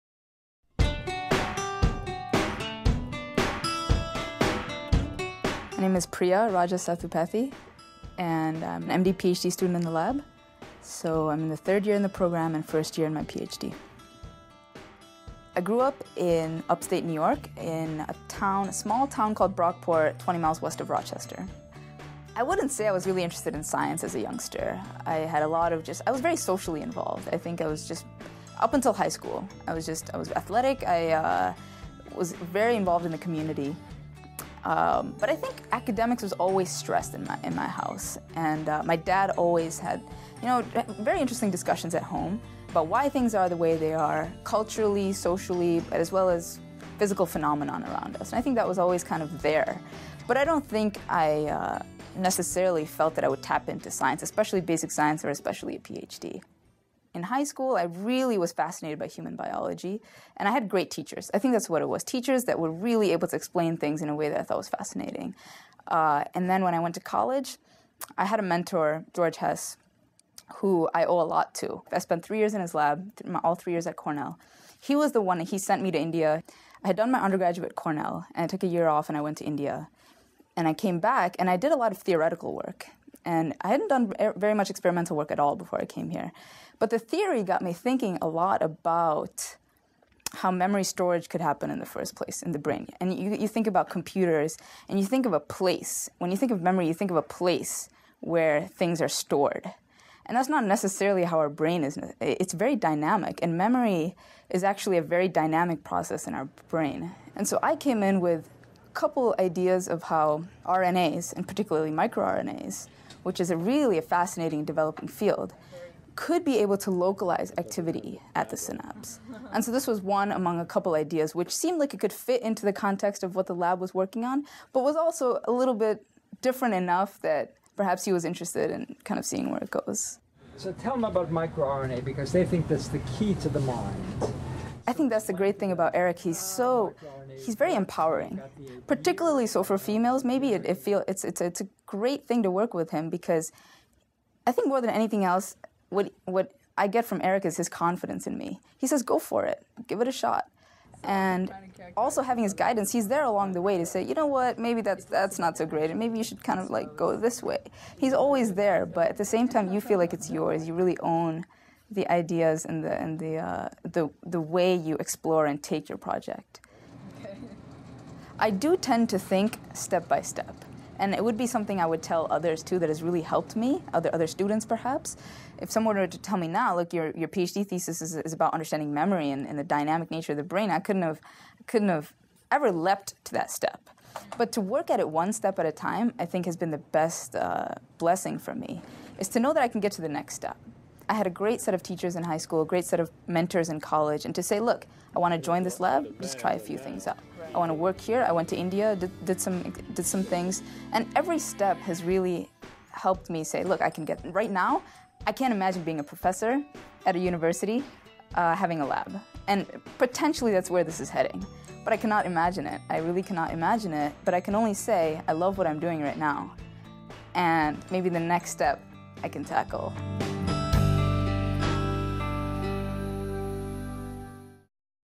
HHMI: Priya Rajasethupathy Interview
An interview with Priya Rajasethupathy, while she was an MD/PhD student in Eric Kandel's lab at Columbia University.

Used with permission from the Howard Hughes Medical Institute, Copyright (2008).